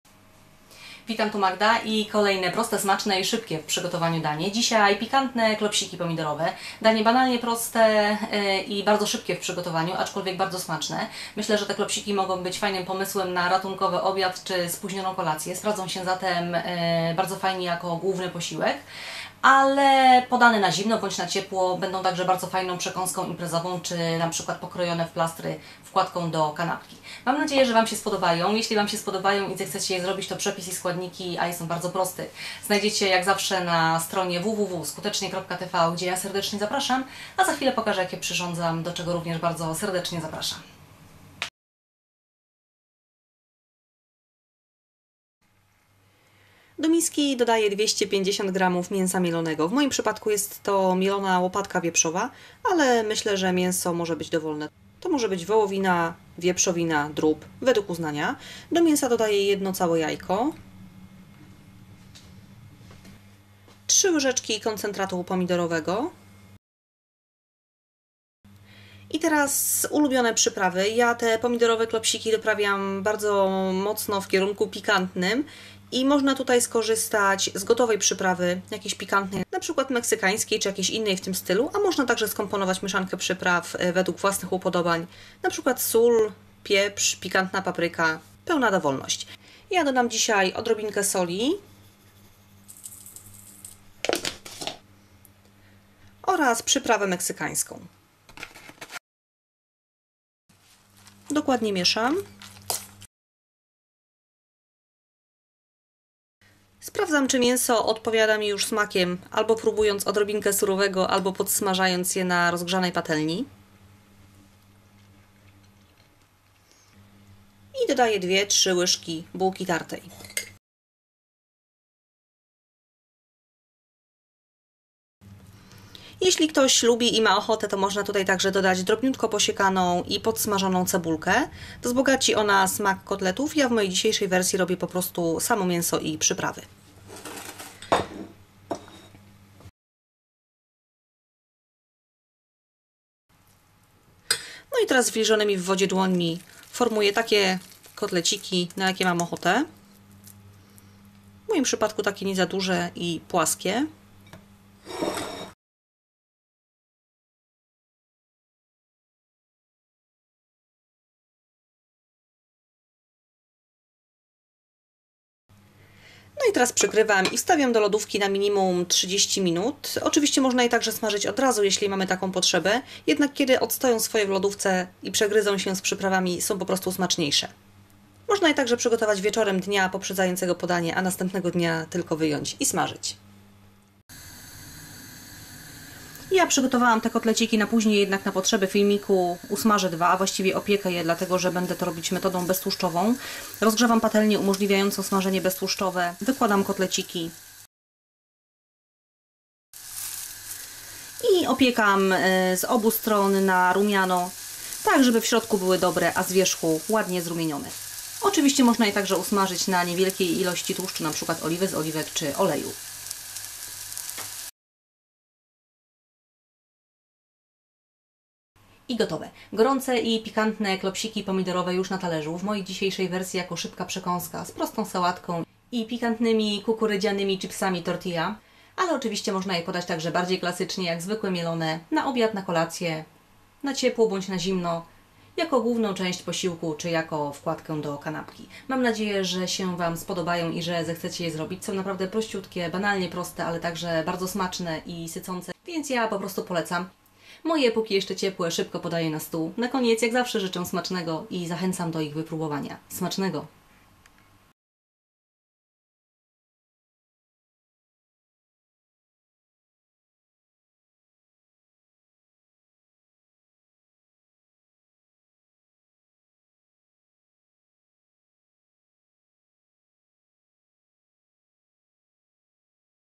Pikantne klopsiki pomidorowe :: Skutecznie.Tv [HD]
Gorące, rumiane, pikantno-pomidorowe... Pyszne mięsne klopsiki, kuleczki lub talarki:) Proste, szybkie i smaczne, a do tego bardzo uniwersalne, bo można  je podać na wiele różnych sposobów -na ciepło, na zimno, jako danie główne, jako imprezową przekąskę czy wkładkę do kanapki. Mega praktyczne i choćby z tego względu warto mieć je w swoim repertuarze:)